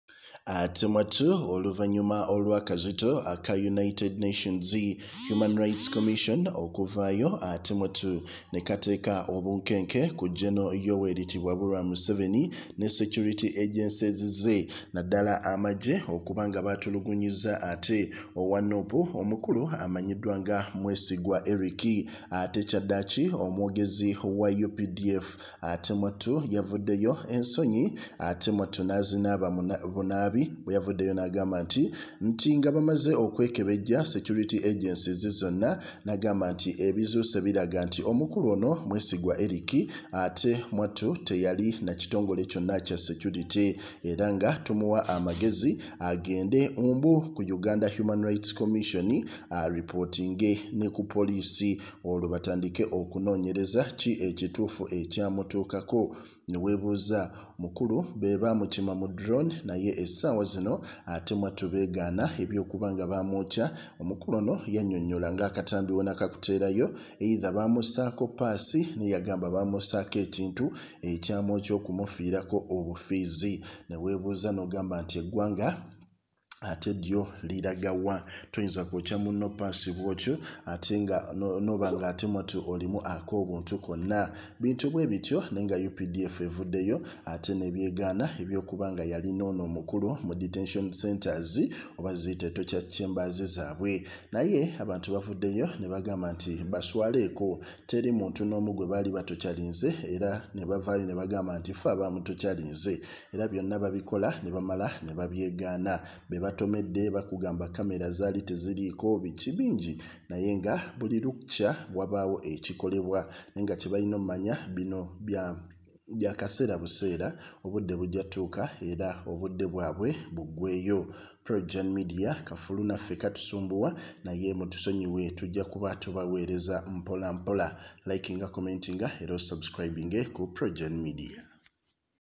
UN tesaaga! Amajje ga museveni Bugakeredde kwebatulugunyiza Owa Bobi wine abaletedde ebizibu
Aganye Okugoba Owa human rights Museveni Alayidde Siwakukikola Wangadya Bobi wine amwekokola
Agobwe! Museveni Bimusobede Okwekalakaasa kutabuse Owa Human rights bagala Agobwe mukifookye giwunye